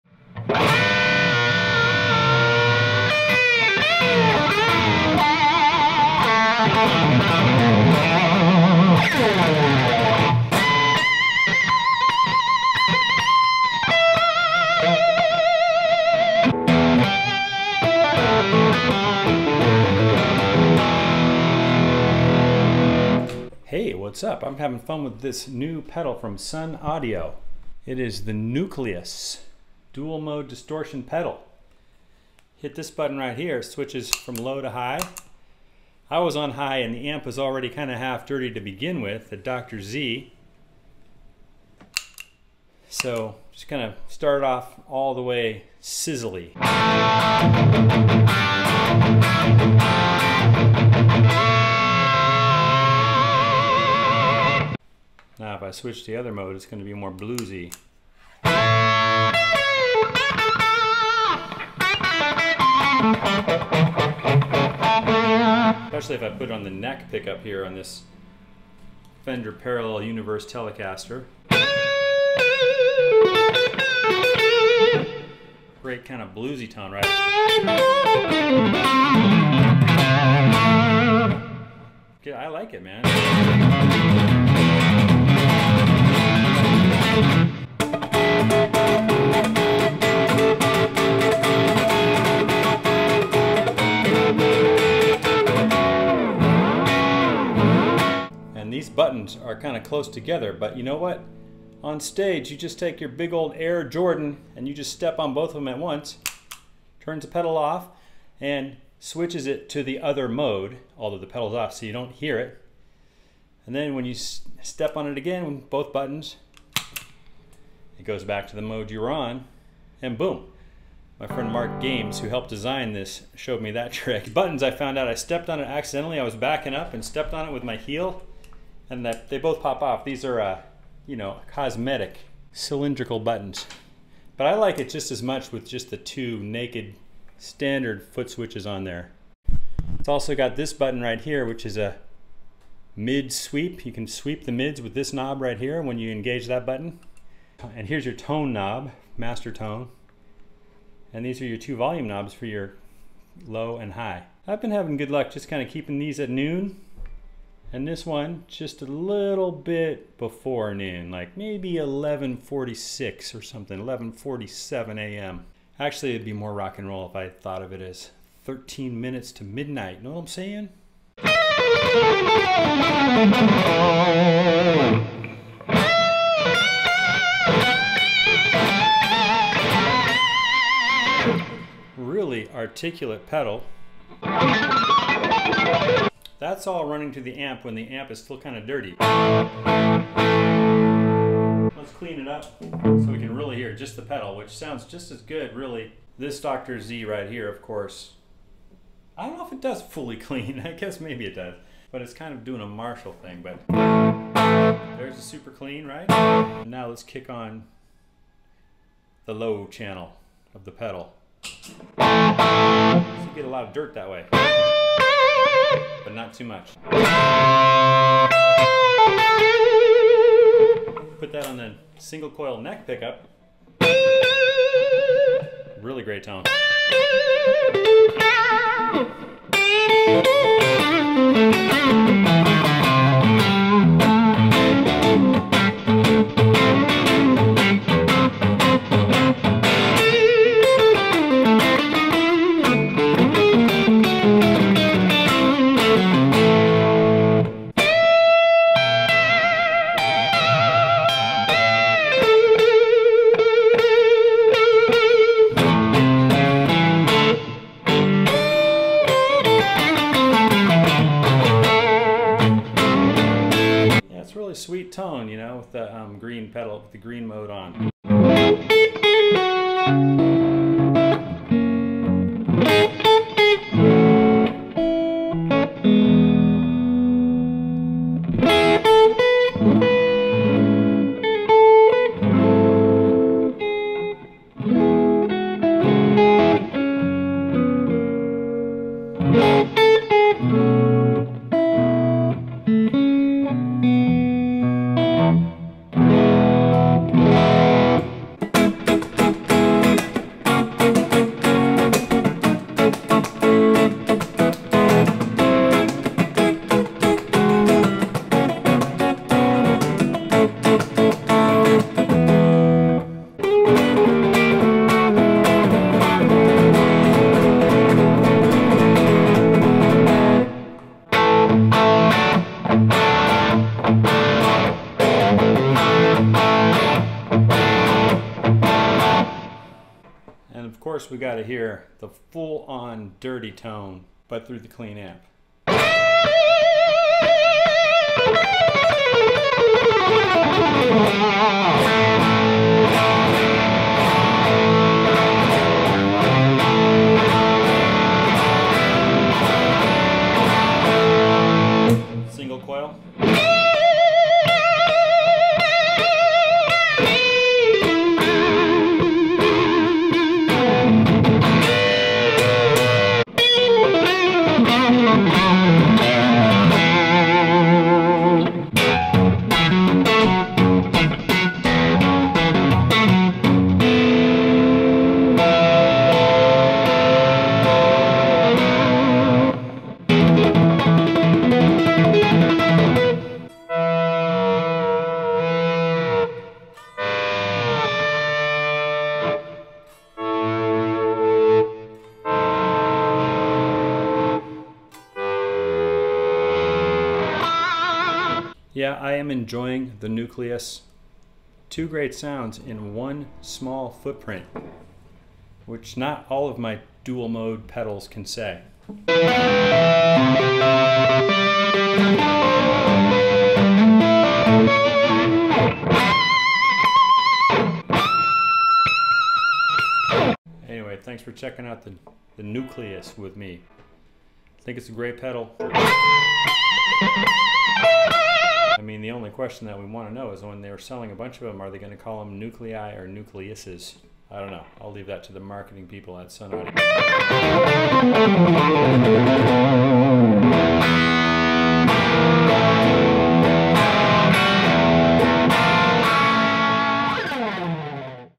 Pedal Test: Sunnaudio Nucleus dual-mode overdrive
Today we plug a Fender Parallel Universe "Nashville" Telecaster through the new Nucleus pedal from Sunnaudio and explore its foot-switchable high- and low-gain modes. Amplifier: Dr. Z EMS head and 2x12 cabinet (which is miked by Royer R-10 and Shure SM-57). The amp's output is attenuated (thus bringing the volume down to a "living room" level) via a Fryette Power Station. No compression or EQ has been added, just a touch of reverb/delay put on there in post.
— JUDE GOLD